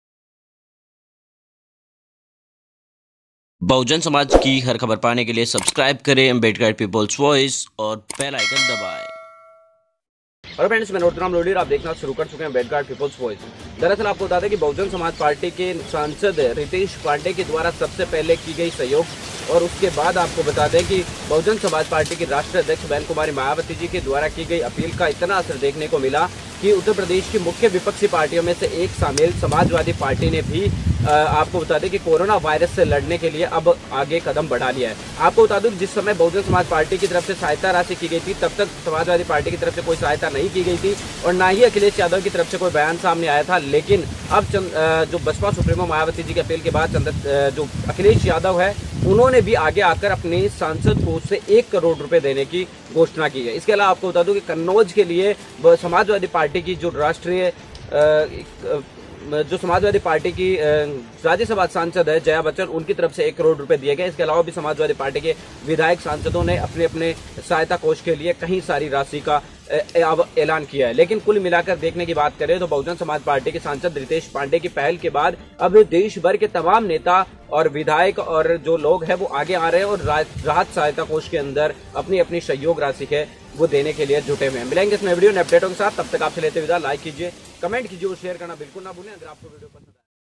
मायावती की अपील के बाद अखिलेश यादव भी देंगे दो करोड रुपये
हमसे जुड़े -